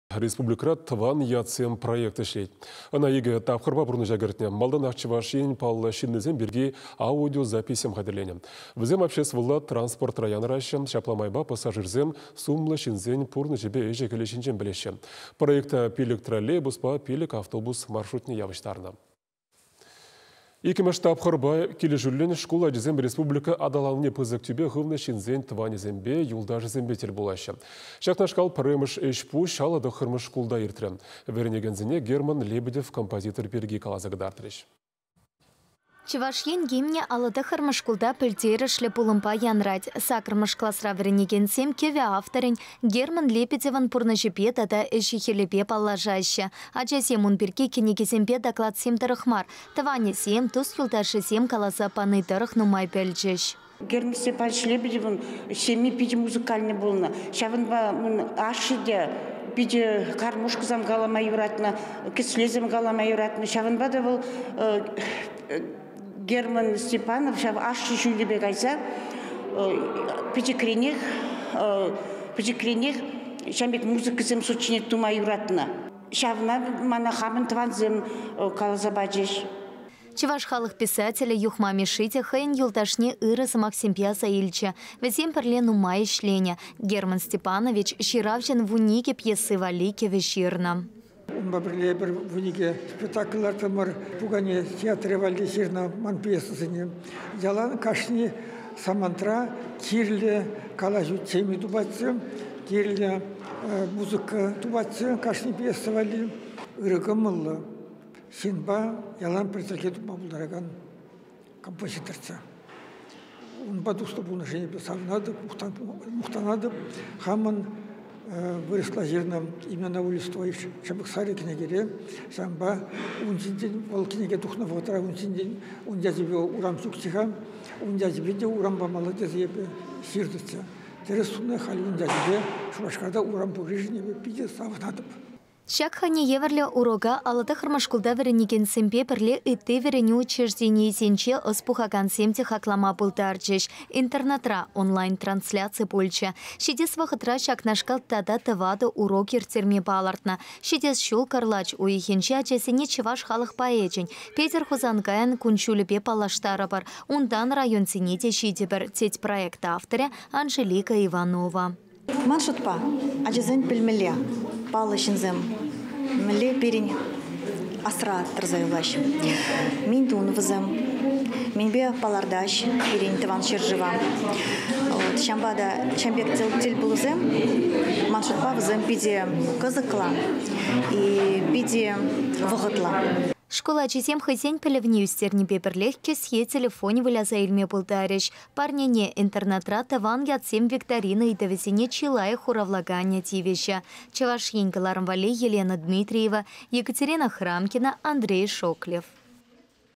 Республикӑра "Тӑван ятсем" проект ӗҫлет
Ӑна икӗ тапхӑрпа пурнӑҫа кӗртнӗ. Малтанах Чӑваш Енӗн  паллӑ ҫыннисем пирки  аудиозаписьсем хатӗрленӗ. Вӗсем обществӑлла транспортра янӑраҫҫӗ, ҫапла майпа пассажирсем сумлӑ ҫынсен пурнӑҫӗпе ӗҫӗ-хӗлӗ ҫинчен пӗлеҫҫӗ. Проекта  5 троллейбуспа 5 автобус маршрутне явӑҫтарнӑ.